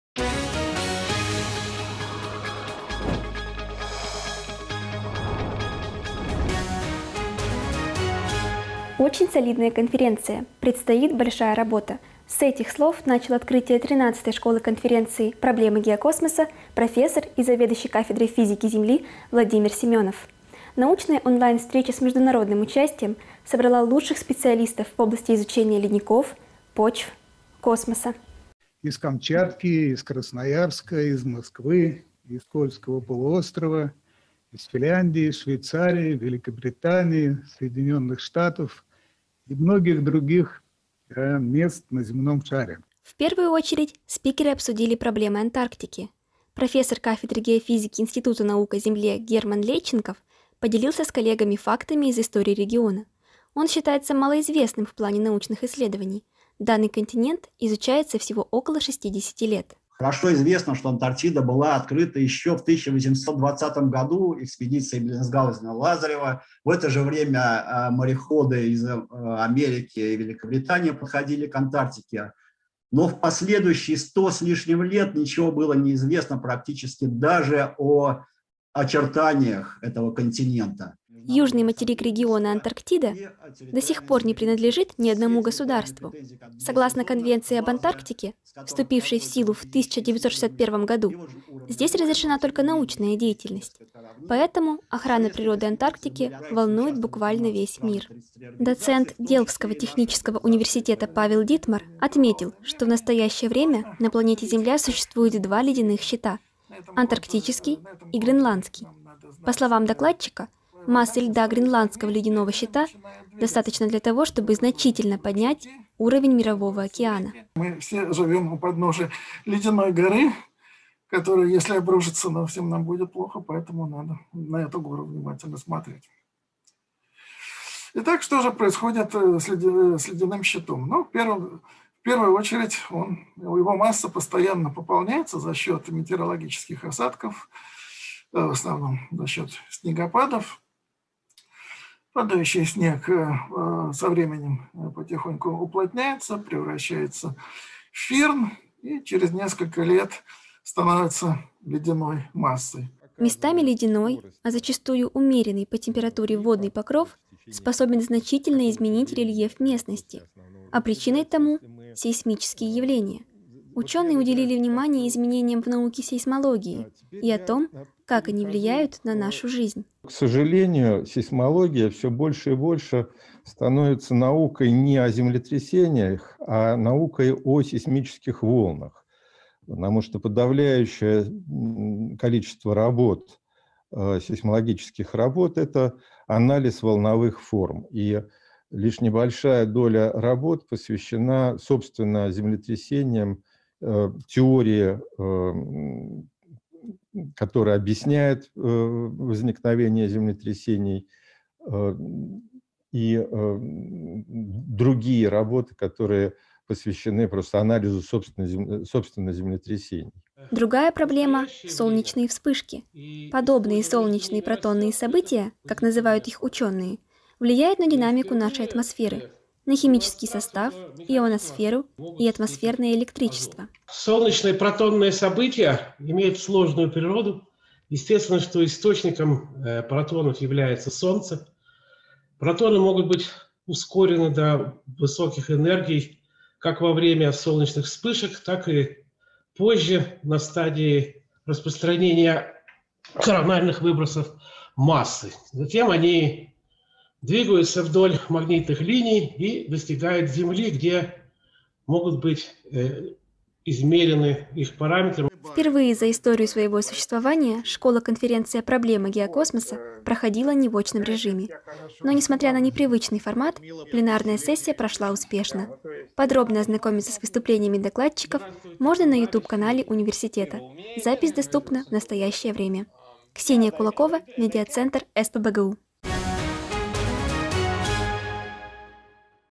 Новости СПбГУ: XIII школа-конференция с международным участием «Проблемы Геокосмоса»
«Солидная конференция. Предстоит большая работа», - с этих слов начал открытие 13-й школы-конференции «Проблемы Геокосмоса» профессор и заведующий кафедрой физики Земли Владимир Семёнов. Научная онлайн-встреча с международным участием собрала лучших специалистов в области изучения ледников, почв, космоса.

Автор - Ксения Кулакова